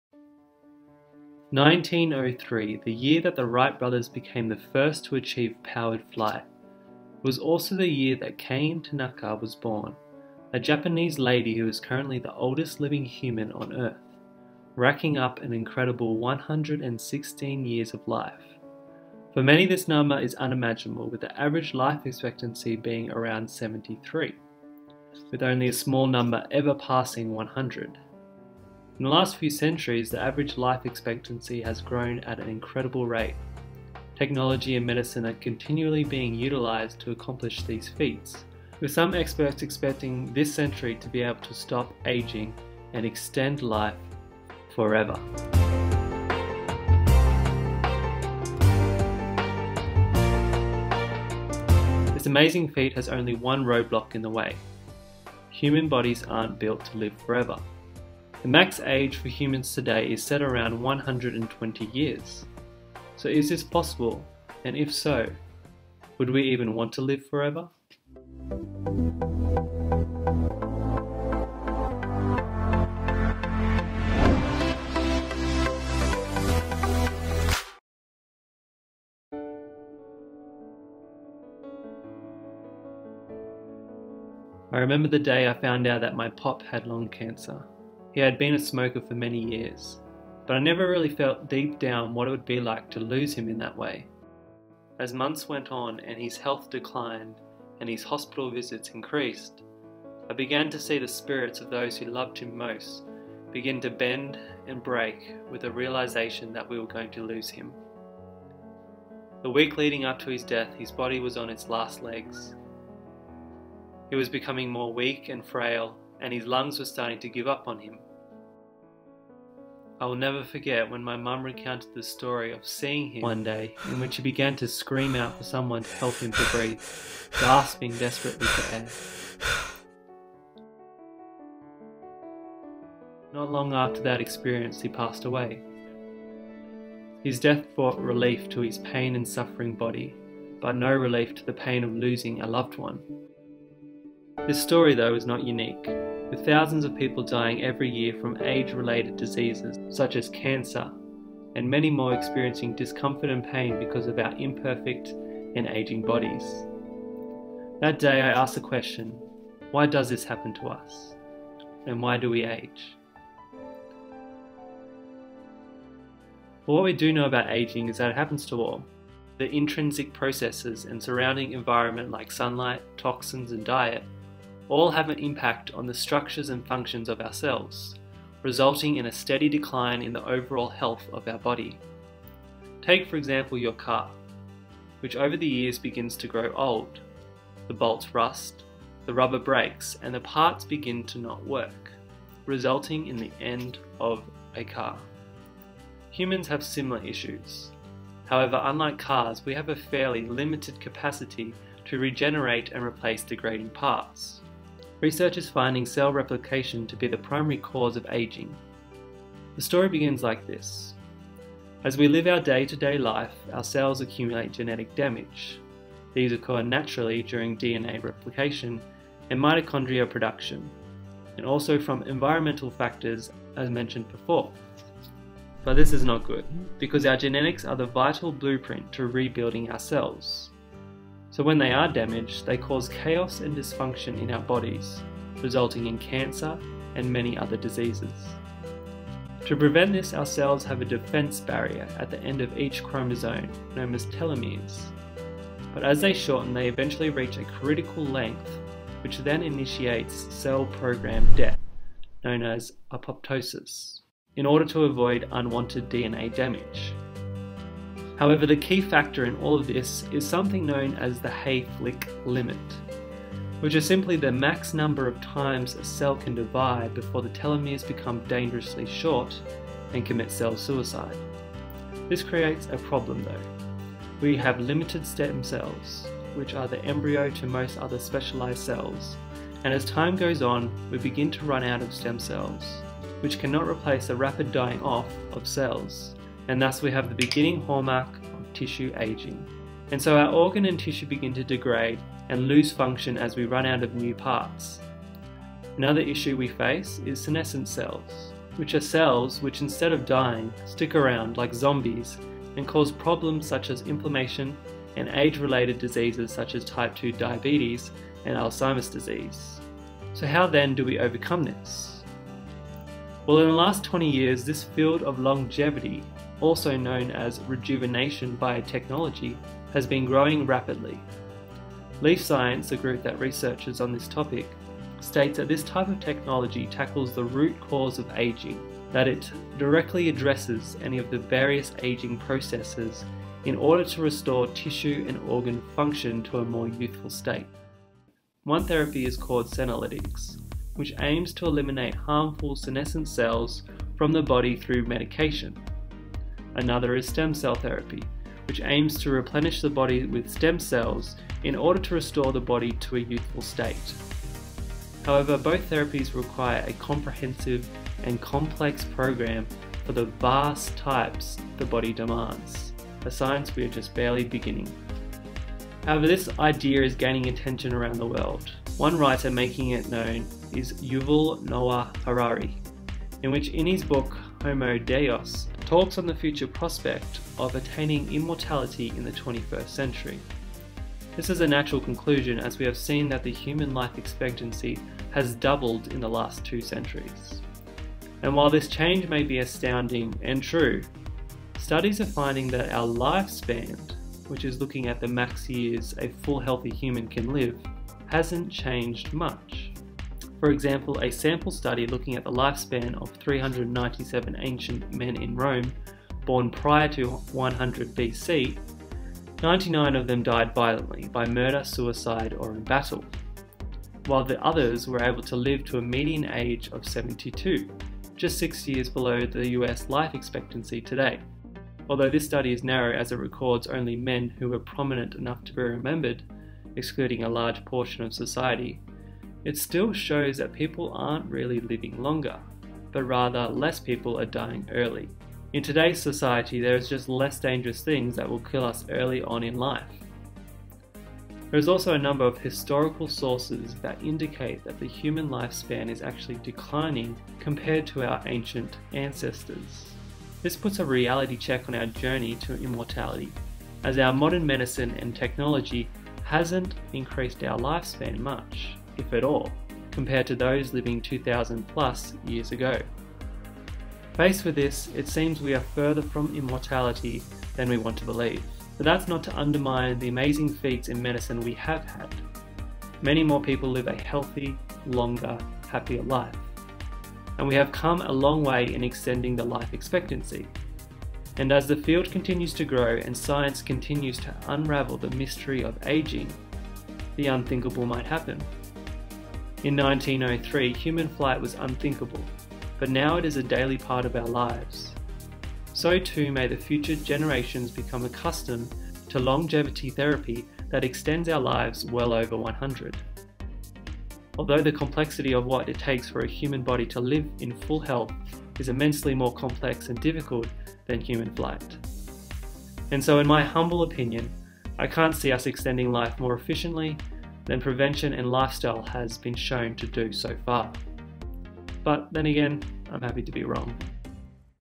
Can We Really Live Forever? Why Do We Age And Can We Stop It?.
What would happen if we could end ageing? Well in this video we are going to look at why we age, how we can stop it and how close are we to reaching immortality! But if we could end aging, would we even want to?...

What is Vitamin B1 (Thiamine) good for? What foods are high in Thiamine (vitamin b1)?

101MRHEALTH